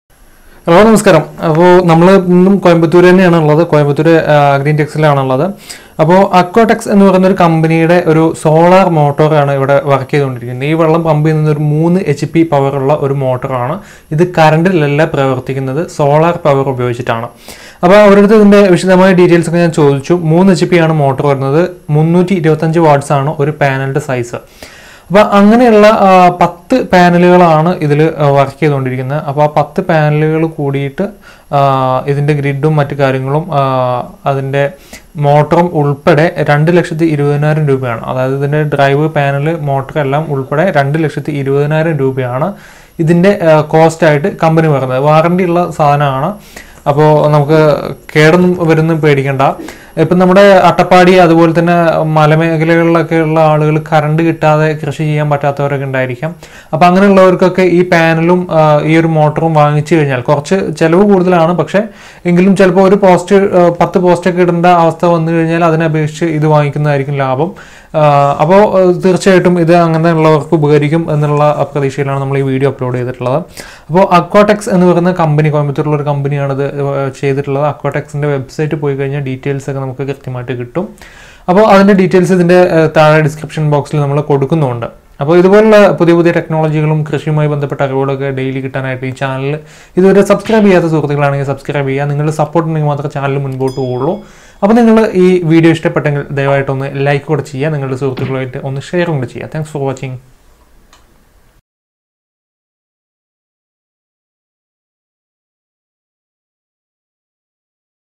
Kseb നീ തീർനെടാ|കരണ്ട് വേണ്ടാ പോസ്റ്റ് വേണ്ട 3hp മോട്ടോർ വർക്ക് ചെയ്യിക്കാം|solar power motor system
In this video iam introducing a revolutionary motor pumping system by aquatex company seethe entire video for best knowledge

With AQUA GROUP's solar pumping systems, farming is no longer restricted to areas connected to the power grid. Farmers can now harness the sun's energy to pump water anywhere. AQUATEX Solar Pumps are designed to start early in the morning and work till late in the evening ensuring maximum discharge per day.